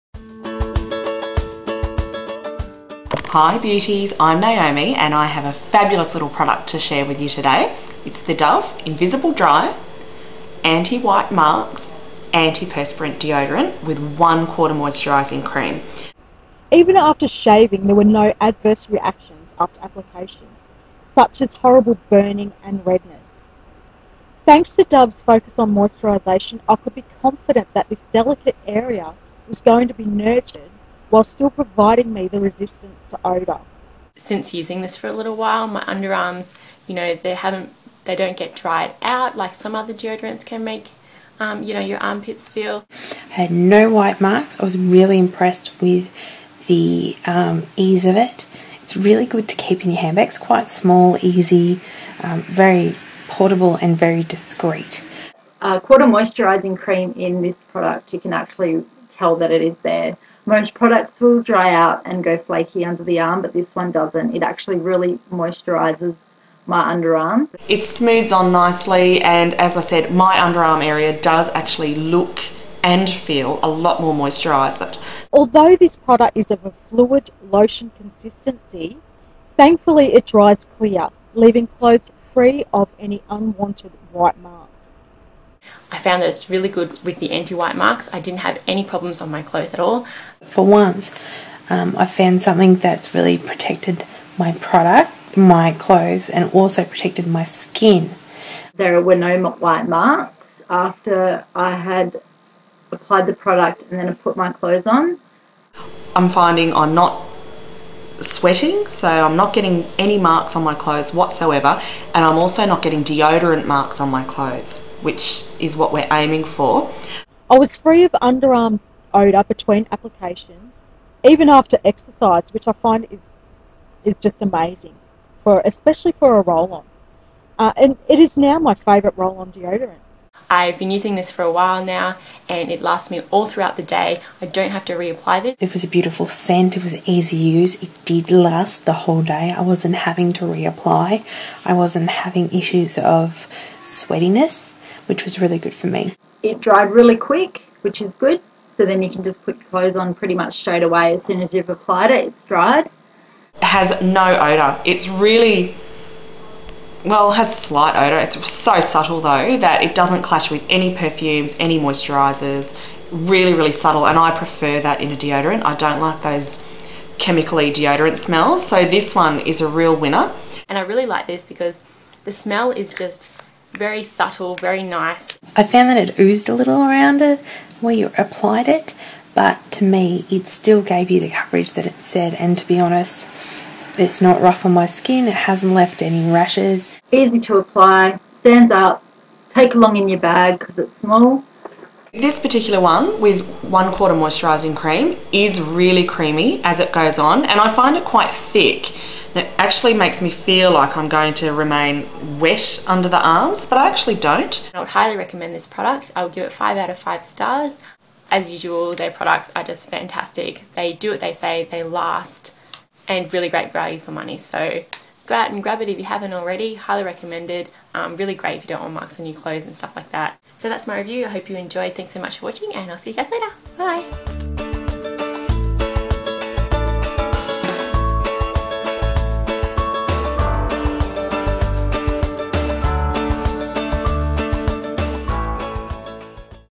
Trial Team Tested: Dove Invisible Dry Anti White Marks
This product could spell the end of annoying white stripes on your clothes...

FEATURED PRODUCTS
Dove Invisible Dry Anti White Marks Roll-on